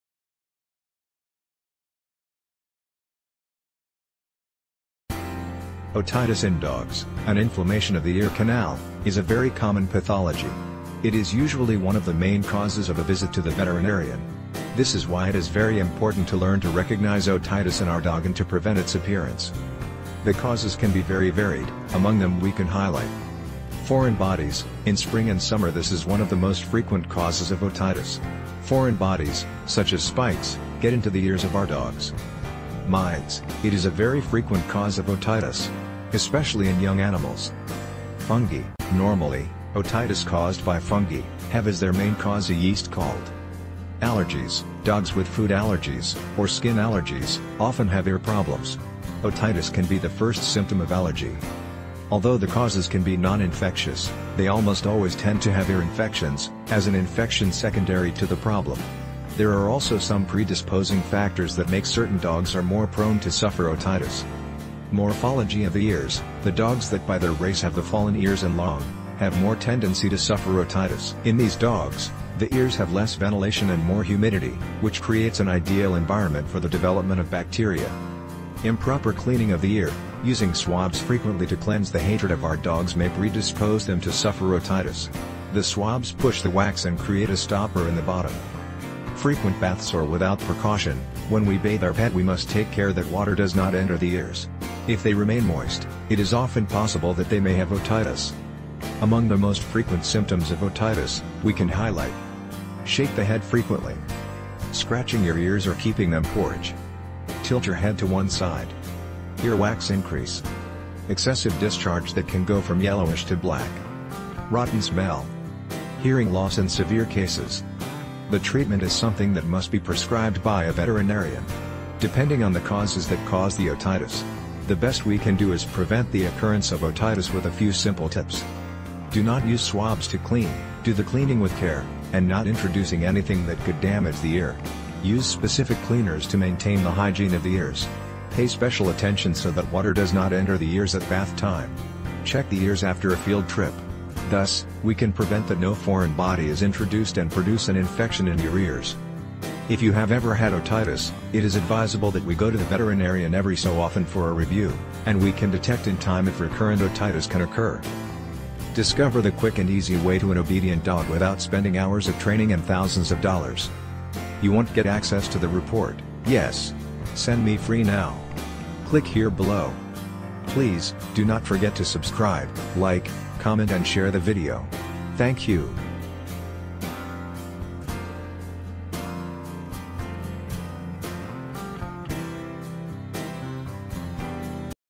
Precautions To Take To Prevent Otitis In Dogs
Otitis in dogs, an inflammation of the ear canal, is a very common pathology. It is usually one of the main causes of a visit to the veterinarian. This is why it is very important to learn to recognize otitis in our dog and to prevent its appearance.

The causes can be very varied, among them we can highlight:

    Foreign bodies: in spring and summer this is one of the most frequent causes of otitis. Foreign bodies, such as spikes, get into the ears of our dogs.

    Mites: it is a very frequent cause of otitis. Especially in young animals.

    Fungi: normally, otitis caused by fungi, have as their main cause a yeast called

    Allergies: Dogs with food allergies, or skin allergies, often have ear problems. Otitis can be the first symptom of allergy.

Although the causes can be non-infectious, they almost always tend to have ear infections, as an infection secondary to the problem. There are also some predisposing factors that make certain dogs are more prone to suffer otitis:

    Morphology of the ears: The dogs that by their race have the fallen ears and long, have more tendency to suffer otitis. In these dogs, the ears have less ventilation and more humidity, which creates an ideal environment for the development of bacteria.

    Improper cleaning of the ear: using swabs frequently to cleanse the hatred of our dogs may predispose them to suffer otitis. The swabs push the wax and create a stopper in the bottom.

    Frequent baths or without precaution: when we bathe our pet we must take care that water does not enter the ears. If they remain moist, it is often possible that they may have otitis.

Among the most frequent symptoms of otitis, we can highlight:

    Shake the head frequently
    Scratching your ears or keeping them porridge
    Tilt your head to one side
    Ear wax increase
    Excessive discharge that can go from yellowish to black
    Rotten smell
    Hearing loss in severe cases.

The treatment is something that must be prescribed by a veterinarian. Depending on the causes that cause the otitis. The best we can do is prevent the occurrence of otitis with a few simple tips:

    Do not use swabs to clean.
    Do the cleaning with care, and not introducing anything that could damage the ear.
    Use specific cleaners to maintain the hygiene of the ears.
    Pay special attention so that water does not enter the ears at bath time.
    Check the ears after a field trip. Thus, we can prevent that no foreign body is introduced and produce an infection in your ears.

If you have ever had otitis, it is advisable that we go to the veterinarian every so often for a review, and we can detect in time if recurrent otitis can occur.